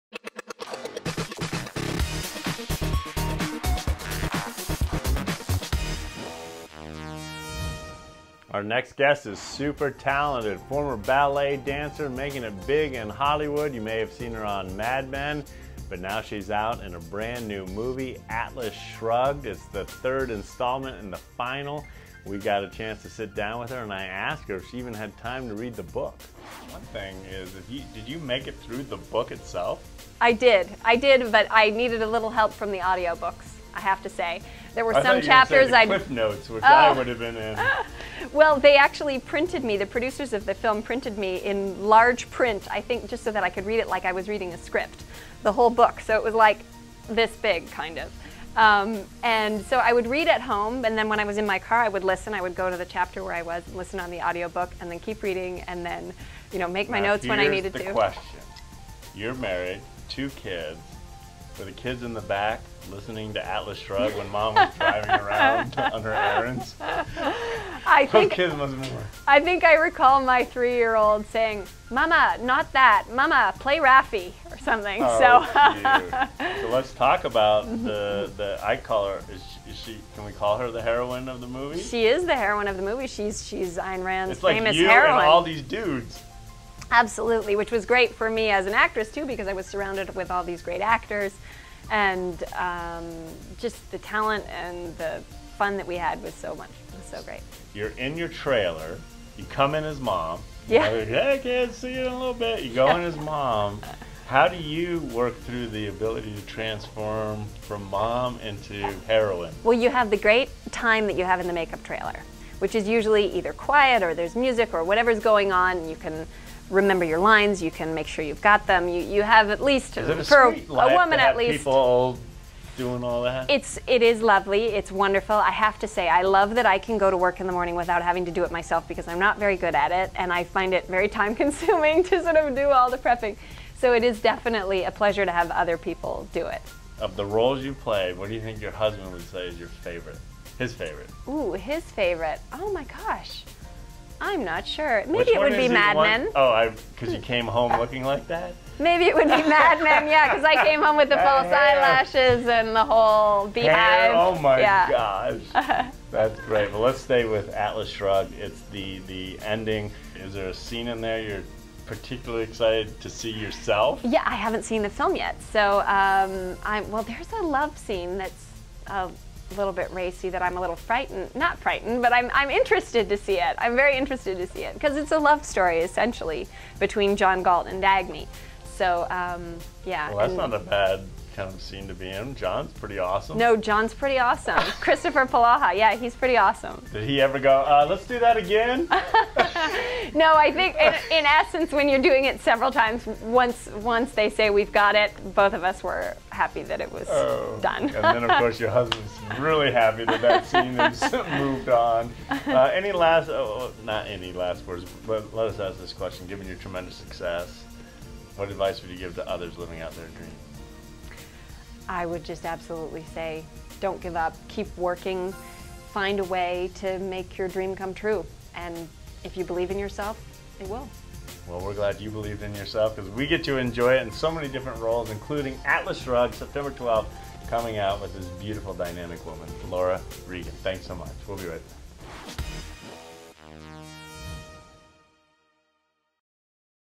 Laura Regan NEW
Laura Regan interviewed by Conn Jackson